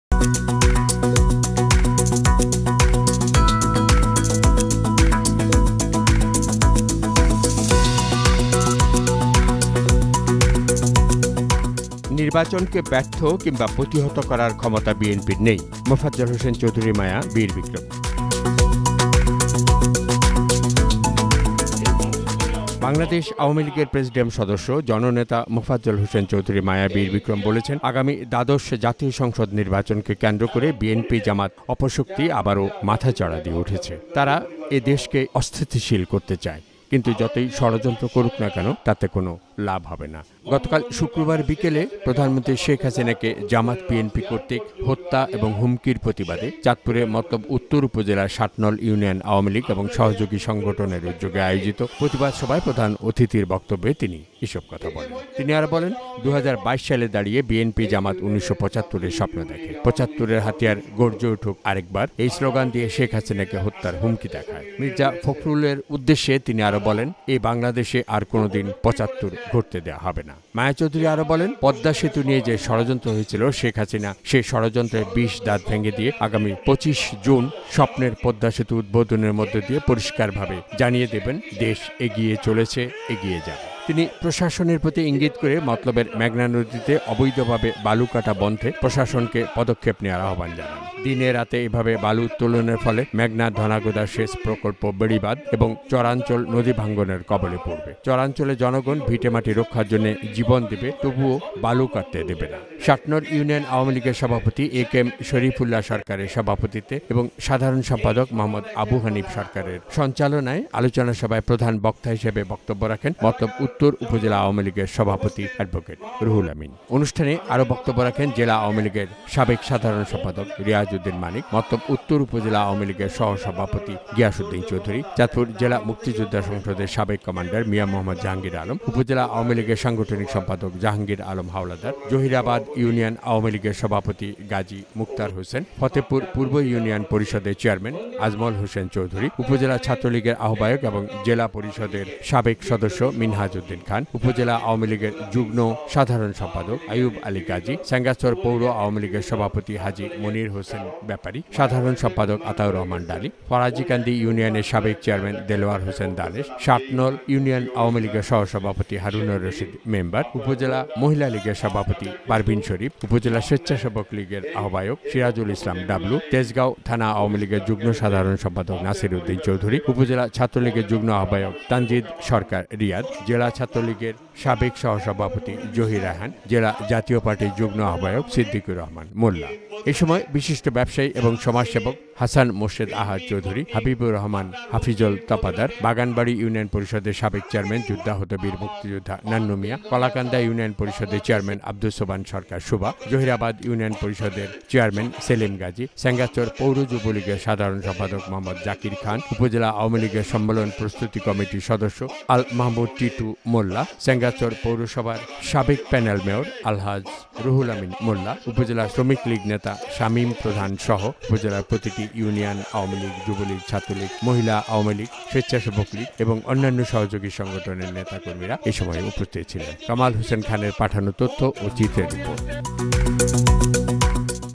নির্বাচনকে ব্যর্থ বা প্রতিহত করার ক্ষমতা বিএনপির নেই -মায়া চৌধুরী
বাংলাদেশ আওয়ামী লীগের প্রেসিডিয়াম সদস্য দুই বারের সাবেক সফল মন্ত্রী চাঁদপুরের কৃতি সন্তান জননেতা মোফাজ্জল হোসেন চৌধুরী মায়া বীর বিক্রম বলেছেন, আগামী দ্বাদশ জাতীয় সংসদ নির্বাচনকে কেন্দ্র করে বিএনপি-জামায়াত অপশক্তি আবারও মাথাচাড়া দিয়ে উঠেছে। তারা দেশকে অস্থিতিশীল করতে চায়। কিন্তু যতই ষড়যন্ত্র করুন কোনো লাভ নেই, নির্বাচনকে ব্যর্থ বা প্রতিহত করার ক্ষমতা আপনাদের নেই।